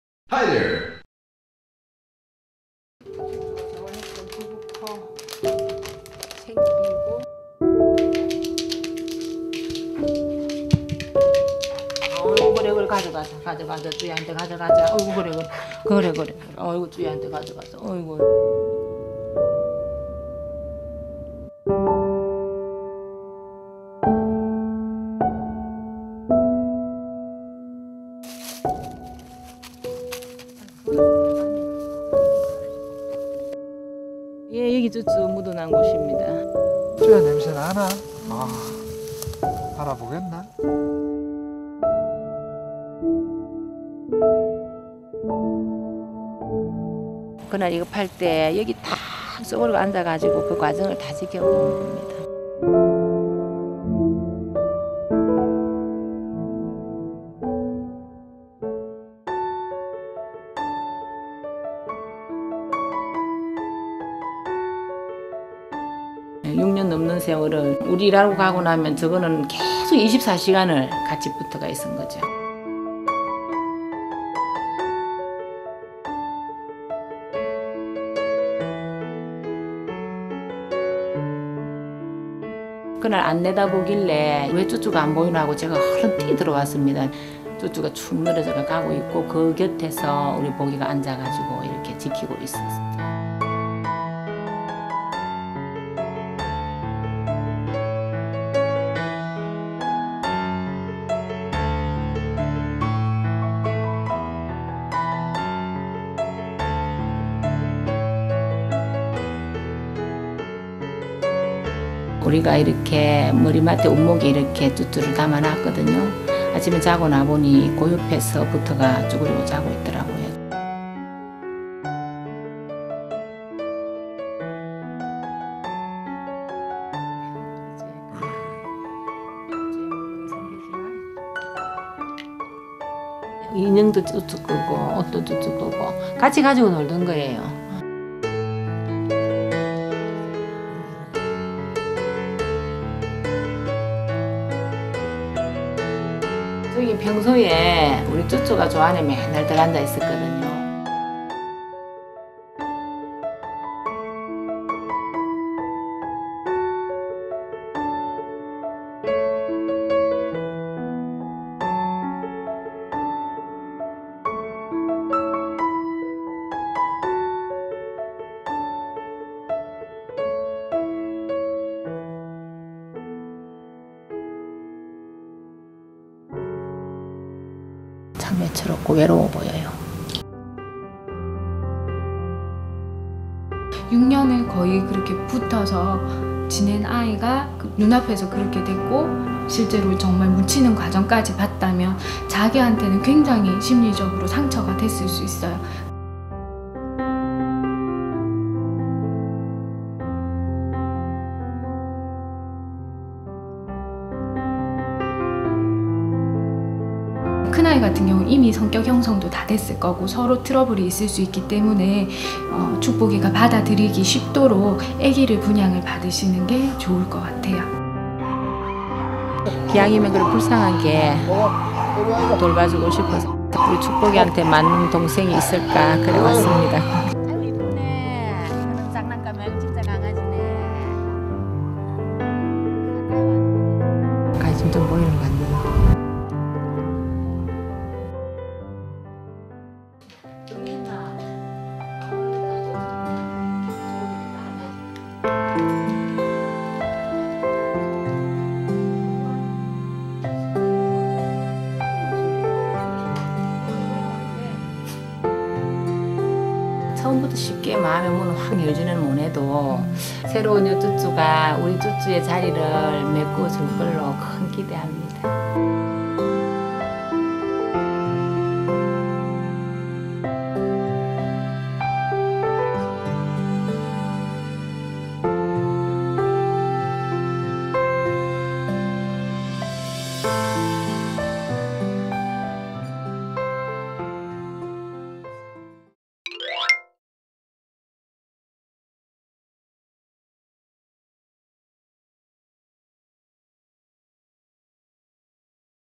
Dog Depressed Over The Loss Of His Best Friend | Kritter Klub
Have you ever missed someone so badly?